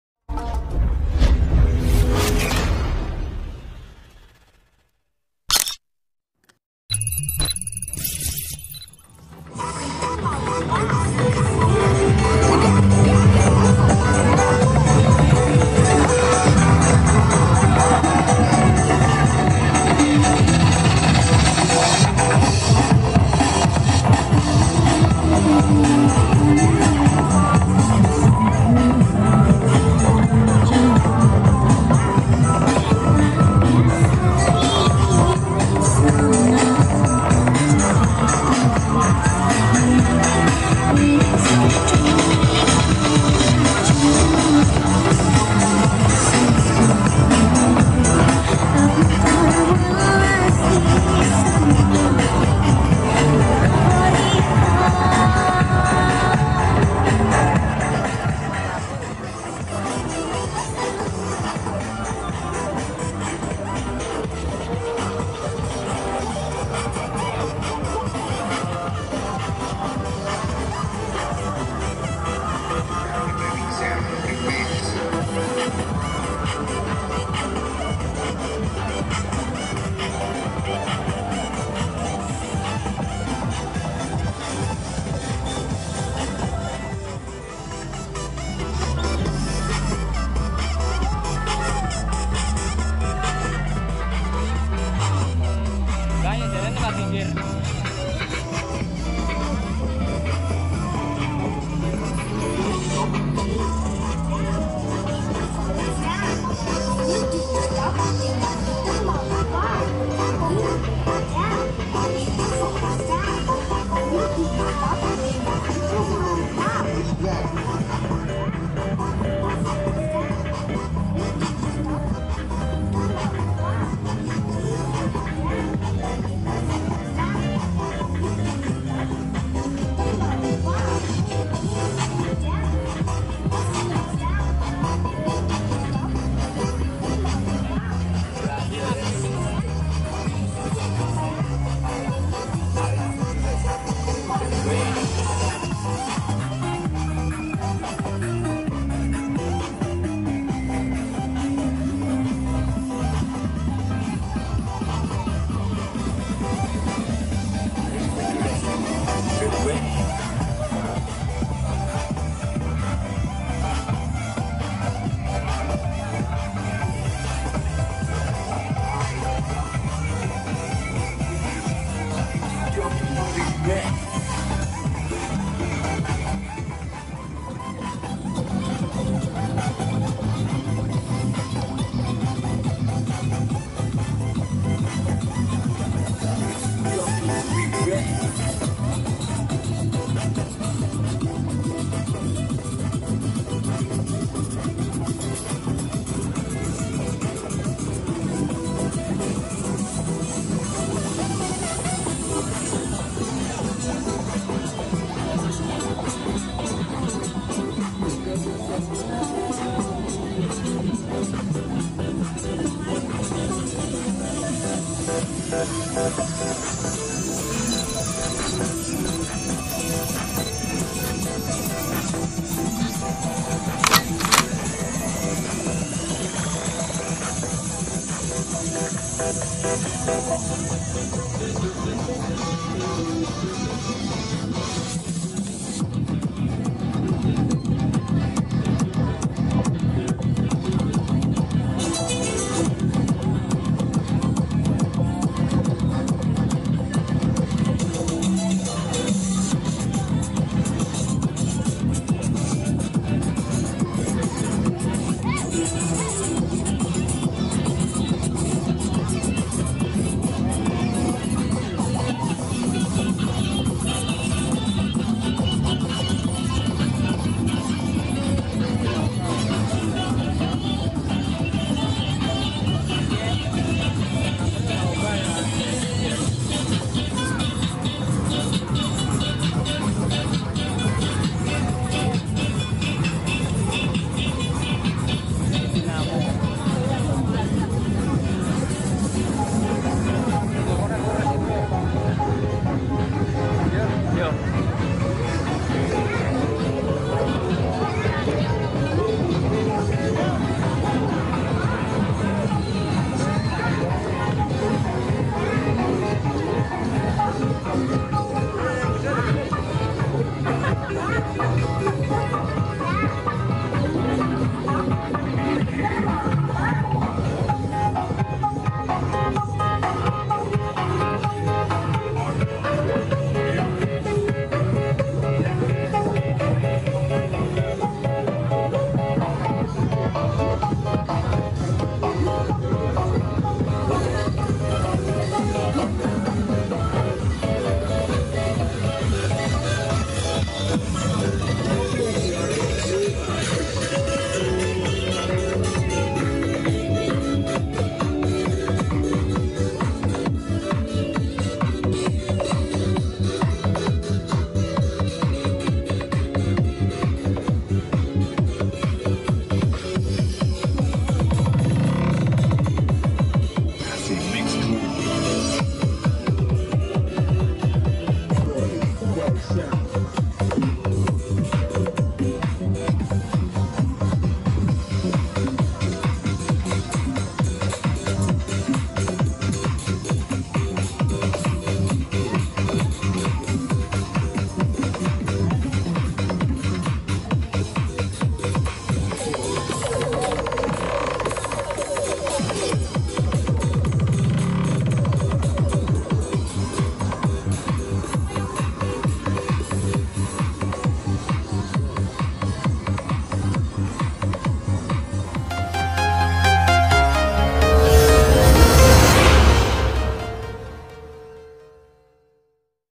Suasana Meriah Pawai Truk Miniatur Penataran Nglegok Blitar
Selamat Datang di Alvinochannel1122, bagaimana kabar Saudaraku semua ? Semoga Sehat Selalu !!
Di kesempatan kali ini saya upload ulang Keseruan karnaval / pawai Miniatur Truk Penataran Nglegok Blitar 2019 Untuk Mengisi Kekosongan acara hiburan Karna Adanya Wabah Covid - 19 dan semoga wabah ini segera selesai dan saya bisa menyajikan acara hiburan audio visual lagi,
Video Dokumentasi ini saya ambil menggunakan Kamera DSLR Edit Kinemaster.
Mohon maaf bila ada kekurangannya, Selamat menyaksikan.